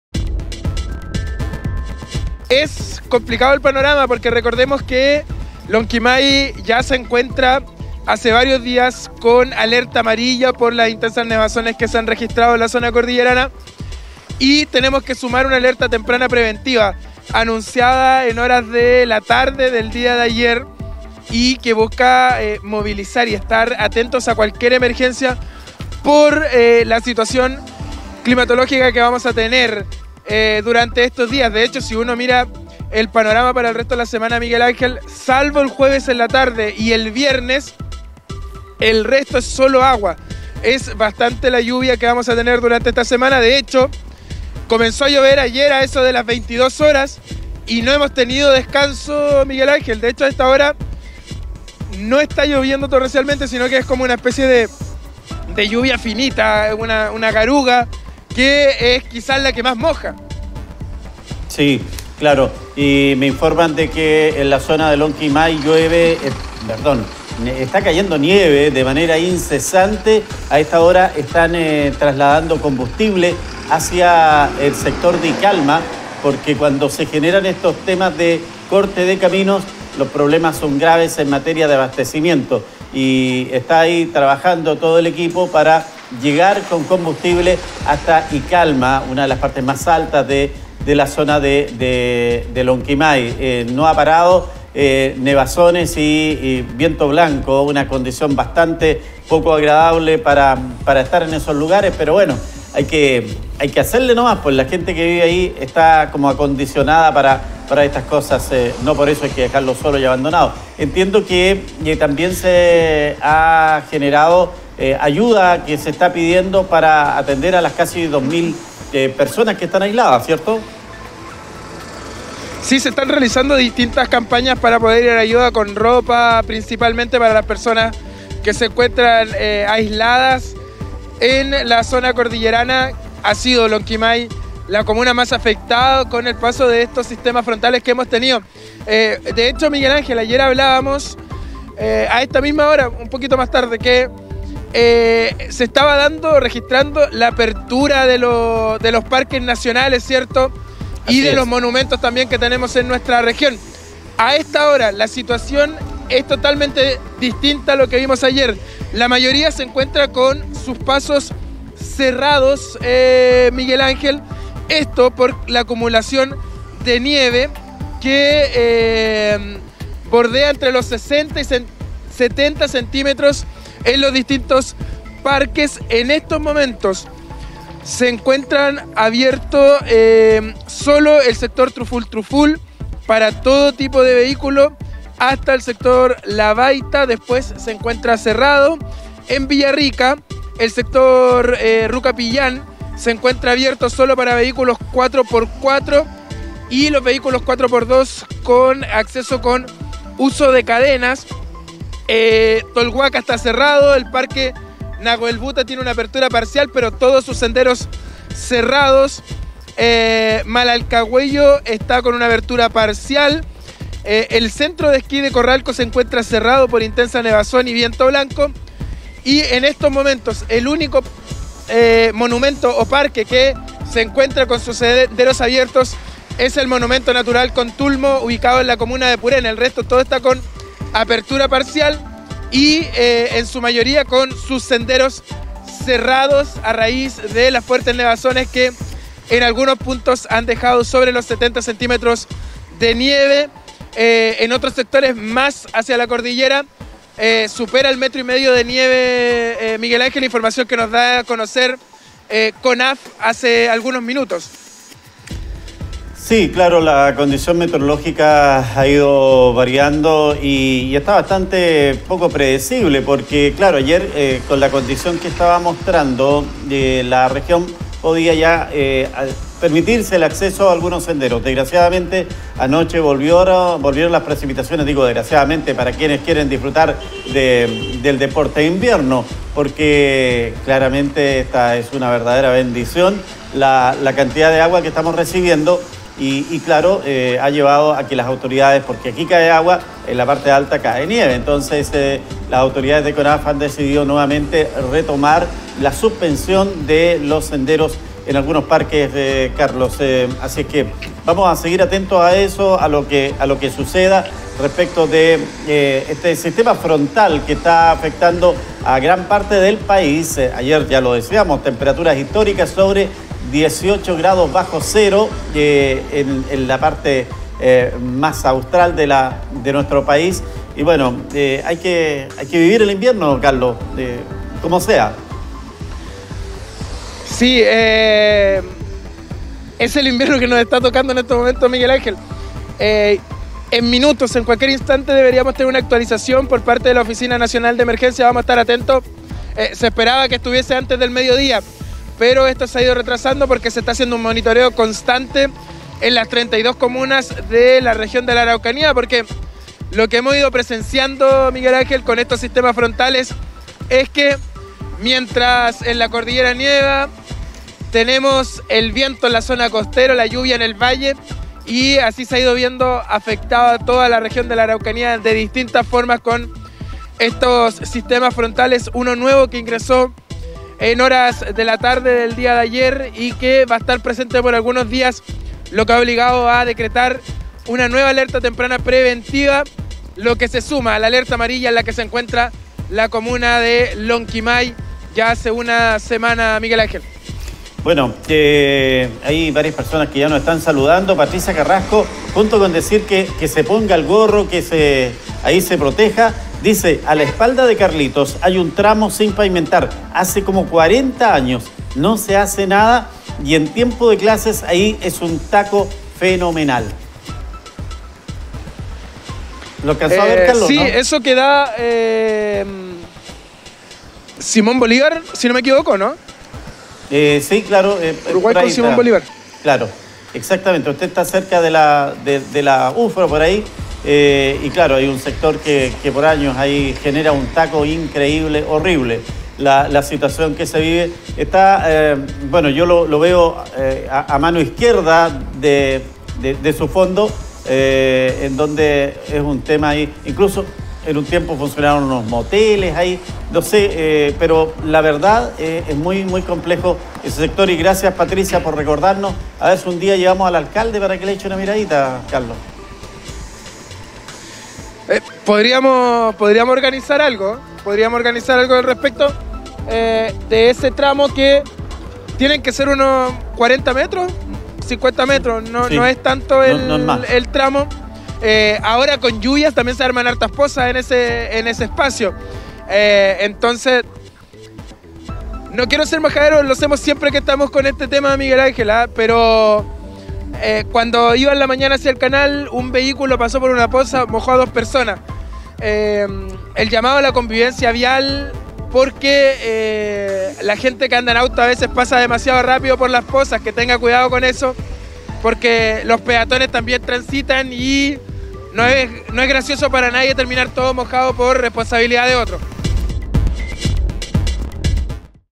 Por sistema frontal declaran Alerta Temprana Preventiva en La Araucanía | ARAUCANÍA 360°
La Oficina Nacional de Emergencia del Ministerio del Interior y Seguridad Pública (ONEMI) ha declarado una Alerta Temprana Preventiva para La Araucanía por sistema frontal con precipitaciones.

La medida fue dada a conocer durante la tarde de este lunes 11 de julio, y tiene como finalidad el movilizar maquinaria y personal para la asistencia en caso de emergencias ocasionadas por las intensas lluvias que se esperan en algunos puntos de la región.

A esto se suma el monitoreo por nevadas que afecta a Lonquimay y que mantienen a la comuna con Alerta Amarilla.